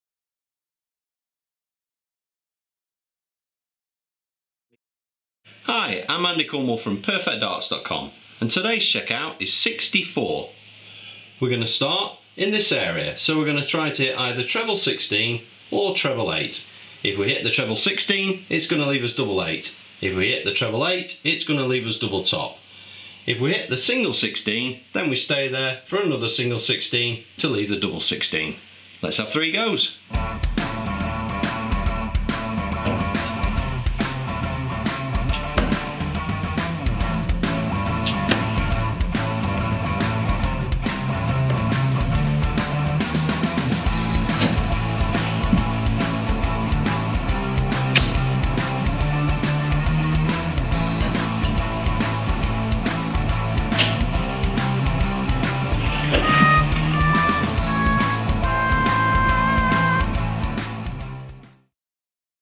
Perfectdarts Darts Checkouts - 64
How to Checkout / Finish The Score of 64.
This Darts Instruction Video by Andy Cornwall is designed to help improve your game. I am an ex Professional Darts Player and Owner of Perfectdarts.com The Online Darts Superstore. I have 3 attempts at the Checkout myself in this video, all of which were shot once only to give a true reflection.